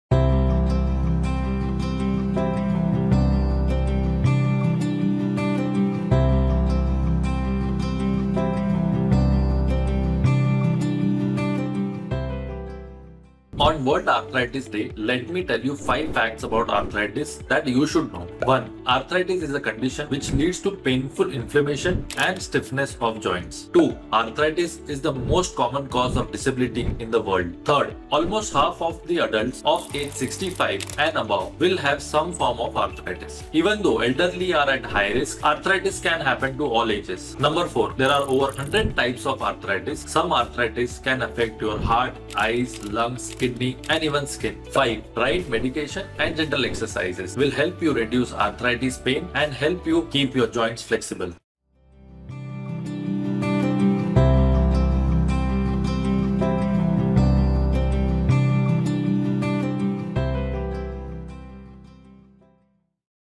Dr. Saijyot Raut | Mumbai | Awareness on Bone and Joint Health | Keep Joints Moving
Mumbai, based Consultant Orthopaedic Spine Surgeon, Dr. Saijyot Raut, shares information about Arthritis in this video of 'Keep Joints Moving,’ an initiative by Pharmed Limited on Bone and Joint Health